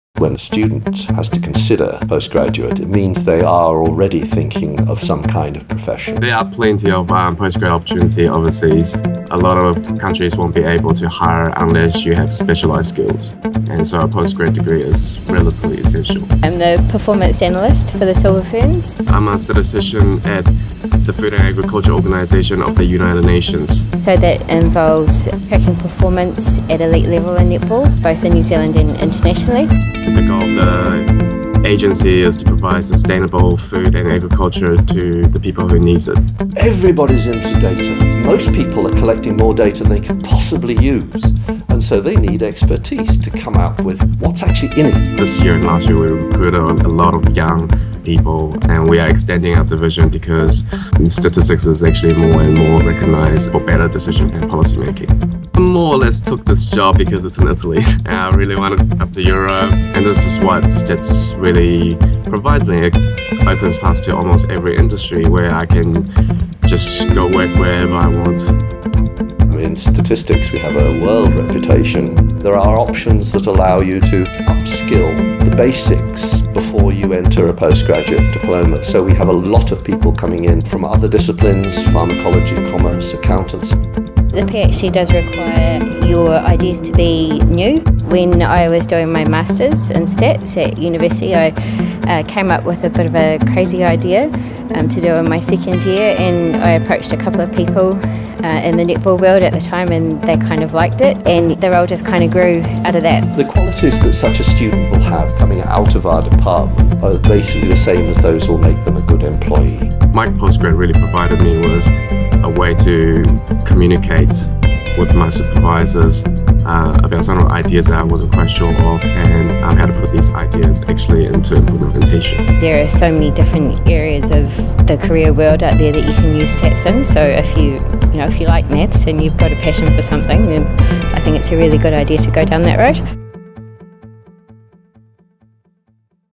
Postgraduate careers in Statistics
Find out where a postgrad degree in Statistics can take you. Bobby Wilcox developed her role with the Silver Ferns by combining her passion for netball with her PhD research. Michael Kao's specialist knowledge and skills are taking him around the world supporting the UN Food and Agriculture Organization.